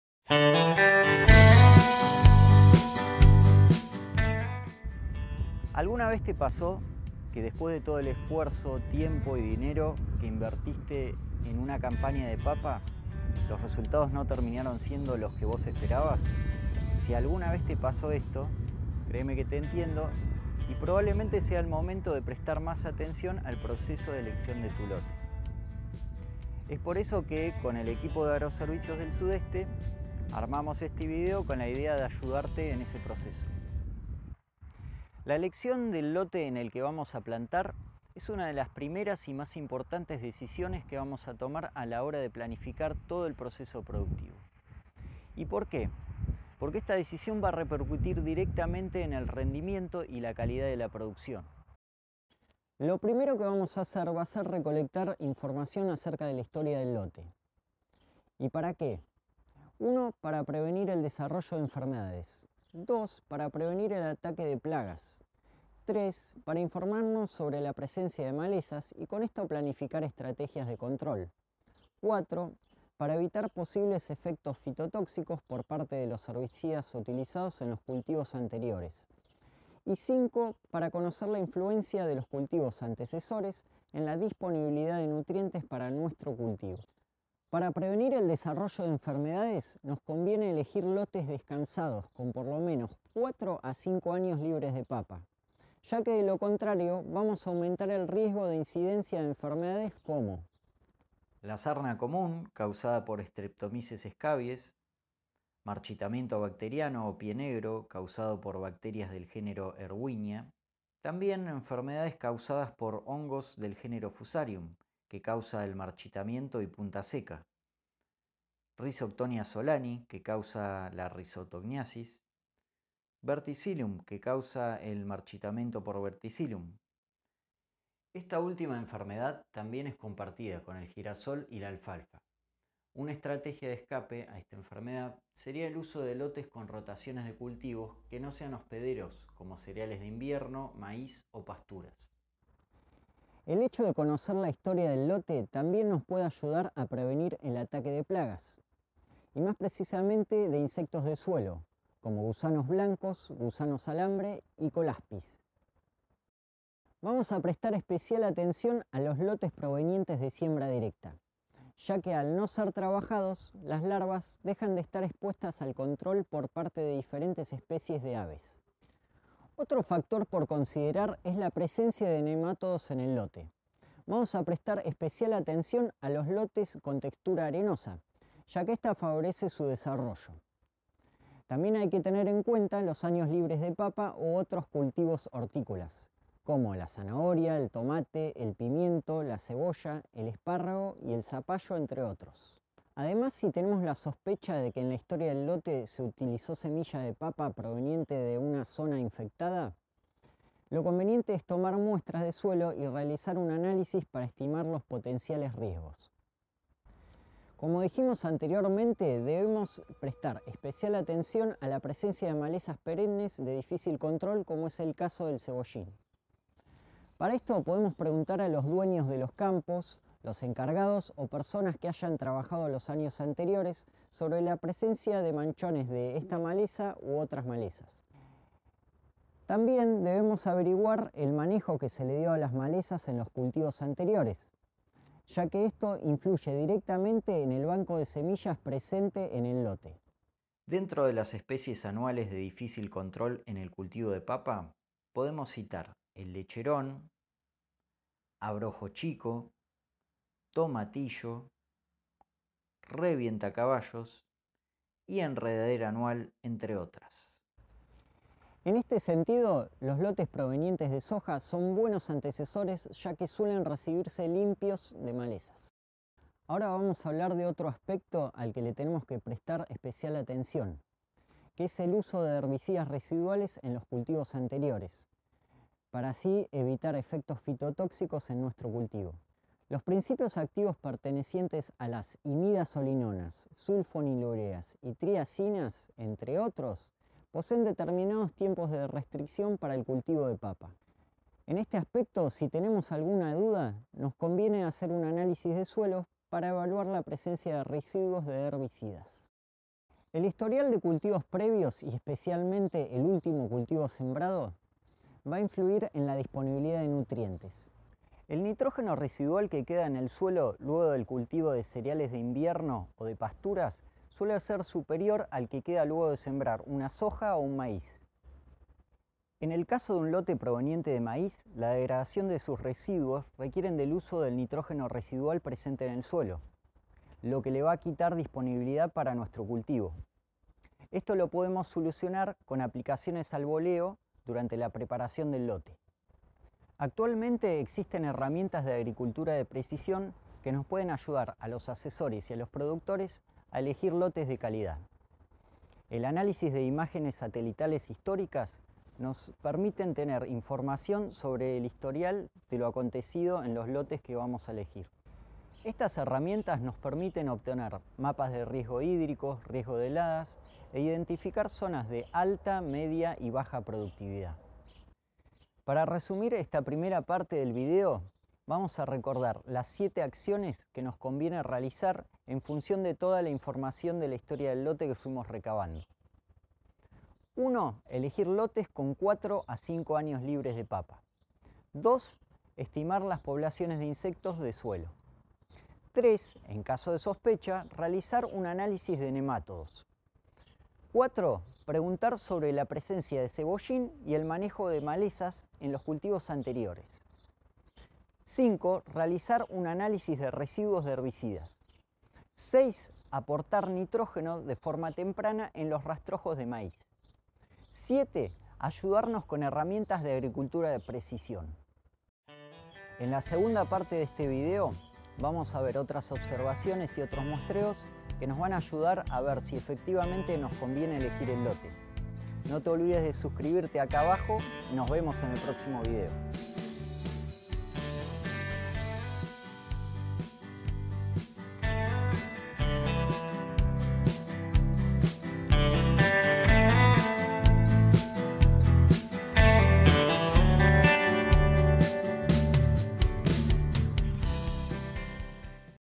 Agroservicios del Sudeste - Elección Del Lote Para Papa (parte 1)
La elección del lote en el cual se realizará el cultivo de papa es una de las primeras y más importantes decisiones a tomar por parte del productor o asesor técnico a la hora de planificar el proceso productivo ya que esta decisión va a repercutir directamente en el rendimiento y calidad de la producción.